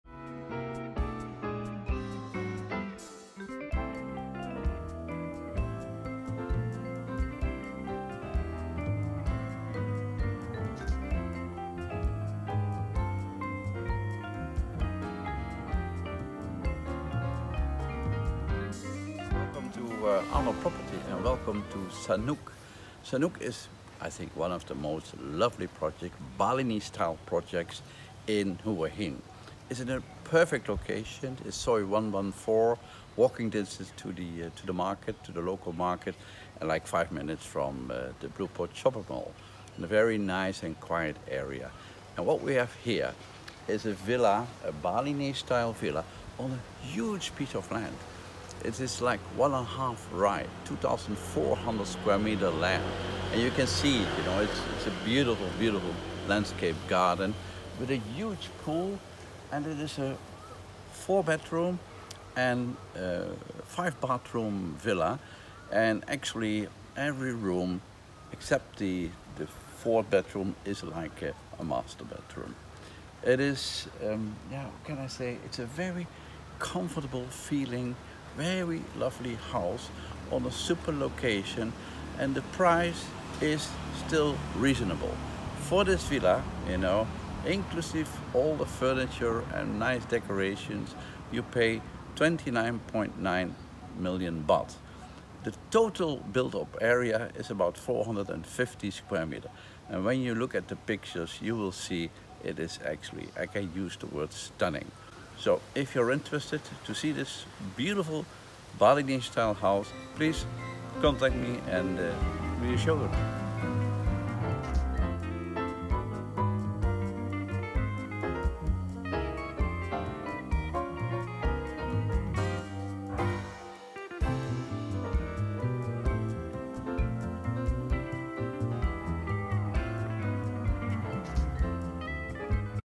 4 Bed Bali-style Luxury Pool Villa for Sale - Soi 114 Hua Hin - THB 29,9 M
- Sanuk Soi 114 Hua Hin
- 4 Bed / 5 Bath
- Land: 2400 m2 / Living: 450 m2
- Price: THB 29,9 M
- Call / WhatsApp Arnold: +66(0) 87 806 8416